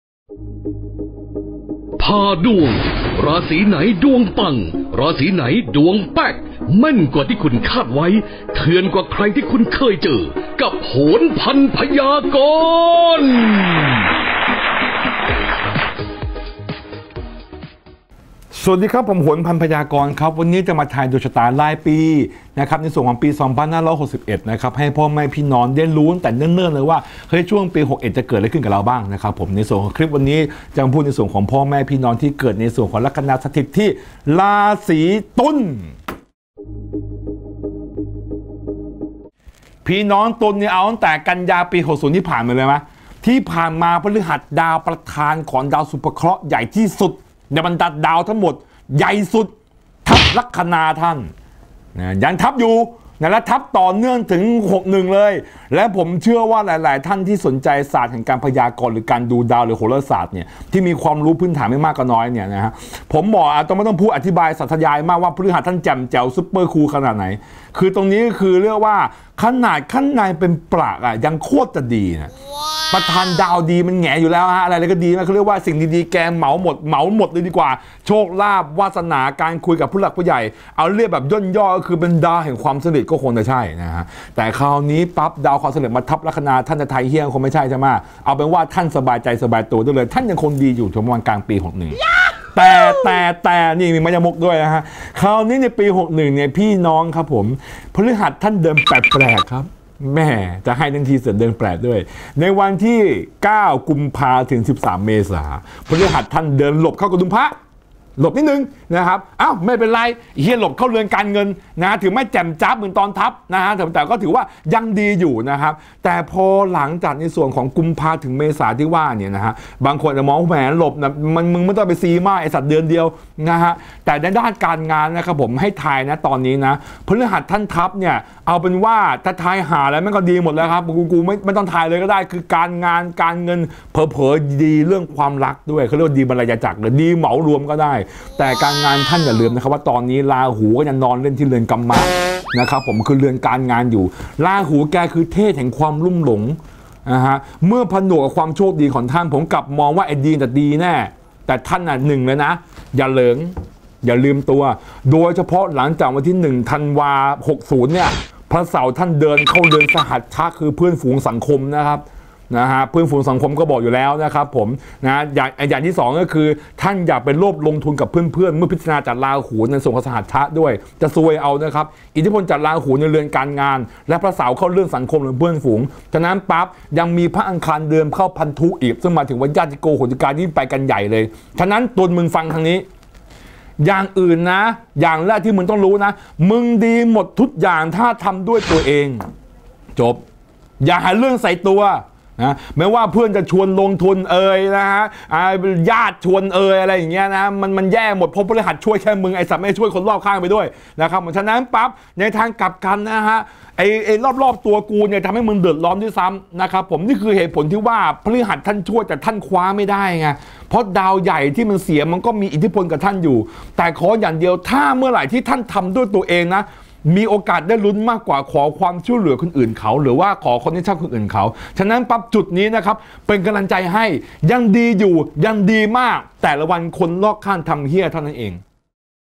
ผ่าดวงปี61 ราศีตุลย์ จะร้ายหรือดีในปีหน้ามาลุ้นกัน !!
มาร่วมลุ้นไปพร้อมๆกัน ว่าราศีของคุณจะมีเรื่องร้าย หรือ เรื่องดีอะไรบ้าง มาฟังจากโหรพันพยากรณ์กันดีกว่า